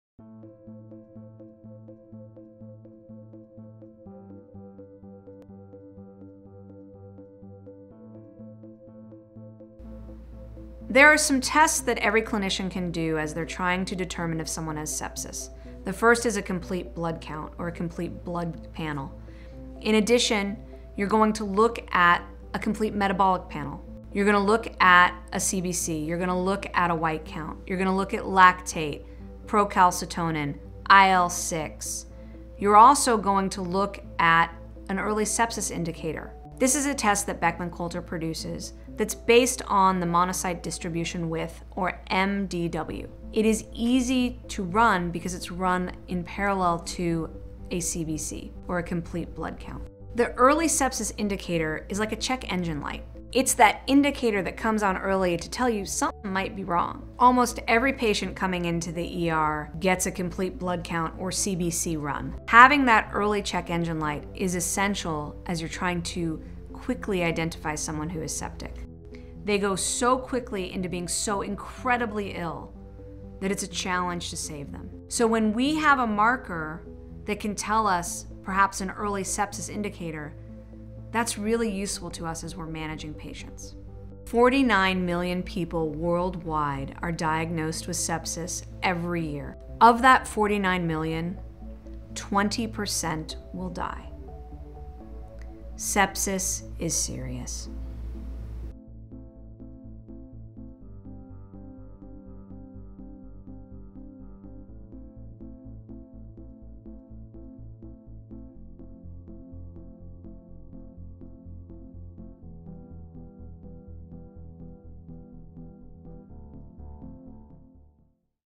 Understanding Sepsis: What tools help diagnose sepsis?
In this video, Shamiram Feinglass, M.D., M.P.H, and chief medical officer at Beckman Coulter, explains the tools that help to diagnose Sepsis.

There are some tests that every clinician can do as they are trying to determine if someone has sepsis. The first is a complete blood count or complete blood panel. In addition, you're going to look at a complete metabolic panel.

You're going to look at a CBC. You're going to look at a white count, you're going to look at lactate, procalcitonin, IL6.

You're going to look for an Early Sepsis Indicator. This is a test that Beckman Coulter produces, it's based on the monocyte distribution width or MDW. It is easy to run because it's run in parallel to a CBC or complete blood count.

The Early Sepsis Indicator is like a check engine light. It's that indicator that comes on early to tell you that something might be wrong. Almost every patient that comes into the emergency room gets a complete blood count or that CBC run.

Having that early check engine light is essential as you're trying to quickly identify someone who is septic. They go so quickly into being so incredibly ill, that it is a challenge to save them. So, when we have a marker, perhaps an Early Sepsis Indicator, that's really useful to us as we are managing patients.

49 million people worldwide are diagnosed with sepsis every year. Of that 49 million. 20 percent will die.

Sepsis is serious.

In the battle to SolveSepsis, every moment counts. For more information about how Beckman Coulter's is helping to solve sepsis, please